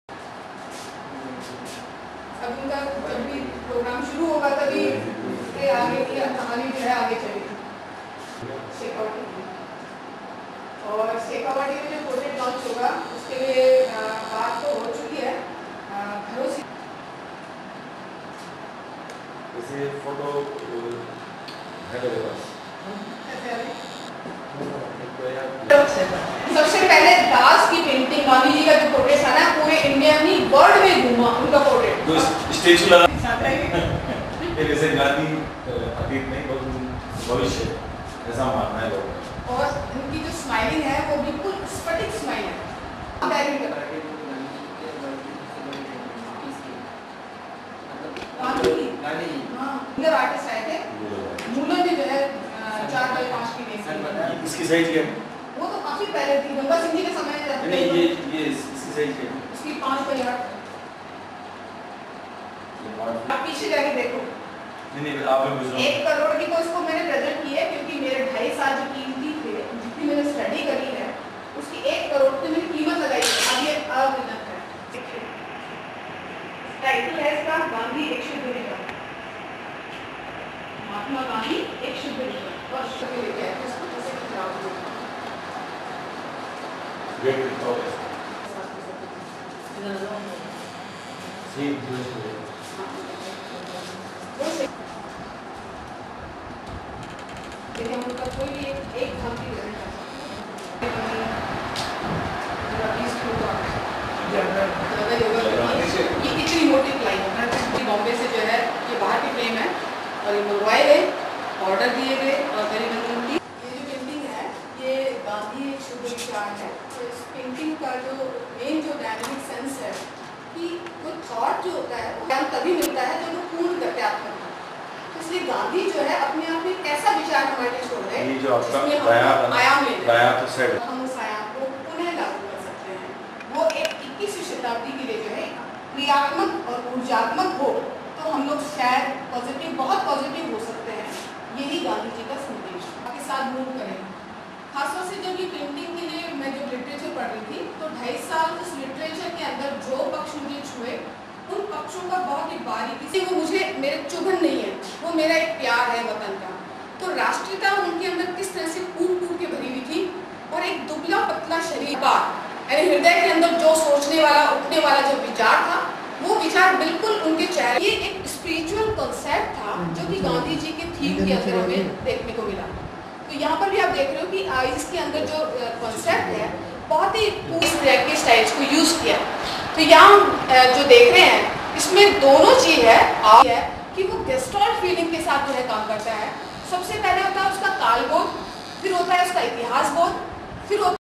DISCOVER TONK SERIES-WORLD LARGEST 3D PAINTING OF MAHATMA GANDHI
SENIOR ARTIST OF VANASTHALI VIDYAPEETH TONK RAJASTHAN MRS PUSHPA DULLAR MADE THIS 3D GANDHI JI'S PAINTING AND SHE CLAIMS THAT IT IS THE LARGEST 3D PAINTING OF MAHATMA GANDHI ALL OVER THE WORLD.